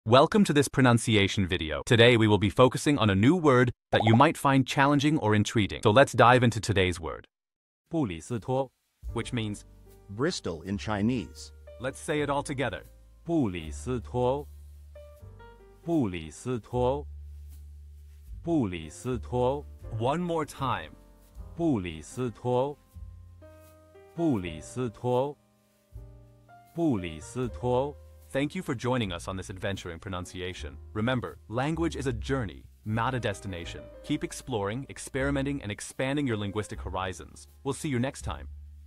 How to pronounce bù lǐ sī tuō | 布里斯托 (Bristol in Chinese)
Master the Pronunciation of 'bù lǐ sī tuō | 布里斯托' - which means : Bristol in Chinese 📝 with @PerfectPronunciationChinese 🎓🗣️ - [Your Guide to Chinese]

Learn to pronounce 'bù lǐ sī tuō' perfectly in Chinese with our easy and engaging tutorial by @PerfectPronunciationChinese! 📖📽️ In this comprehensive video, we provide you with native speaker examples 🗣️👂 and phonetic breakdowns 🎵 to ensure you get the pronunciation just right.